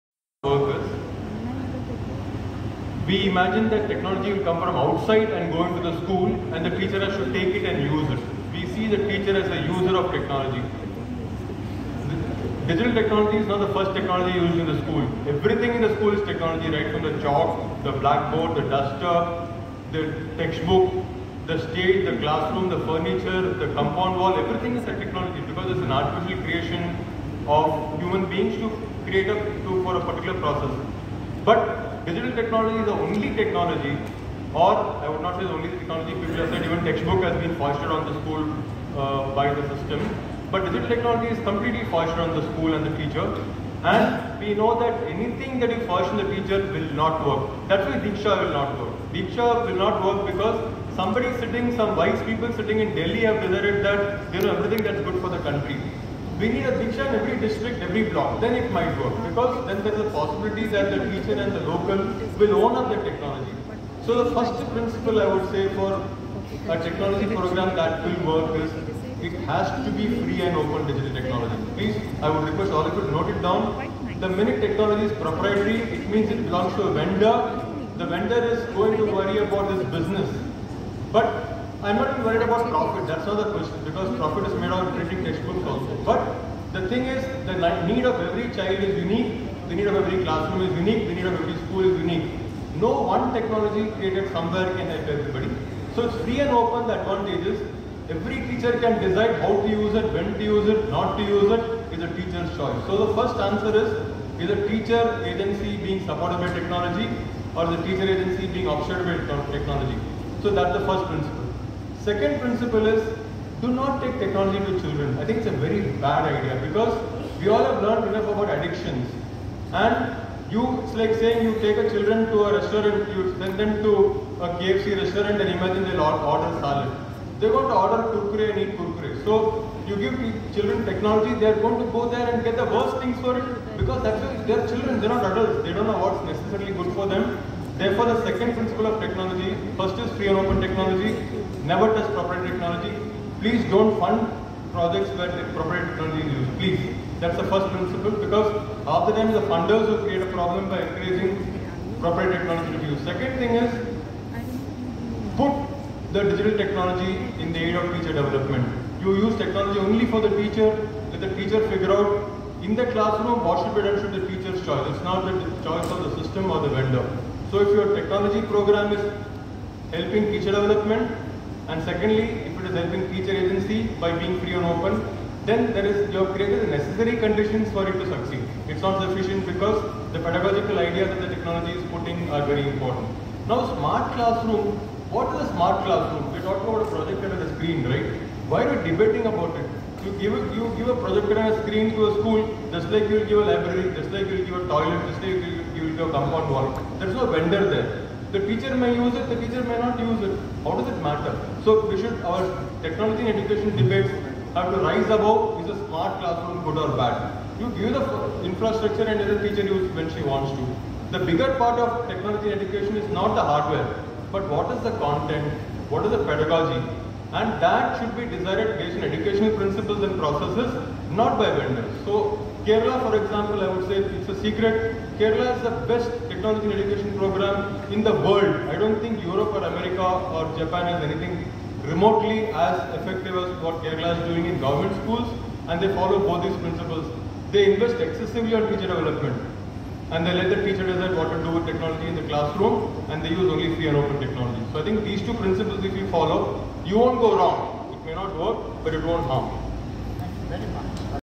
Gurumurthy Kasinathan at RTE Forum - How Kerala EdTech model has worked, necessary for RTE
Gurumurthy at RTE Forum -April 1, 2024 at IIC. Organized by RTE Cell, CSD
How Kerala EdTech model has worked, and it is necessary for achieving RTE